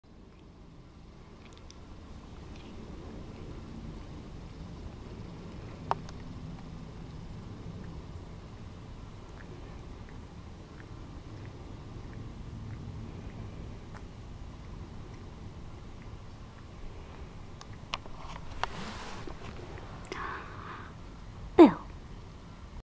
Macy's a princess and gets her own chair
Macy in her first year -- 2010/2011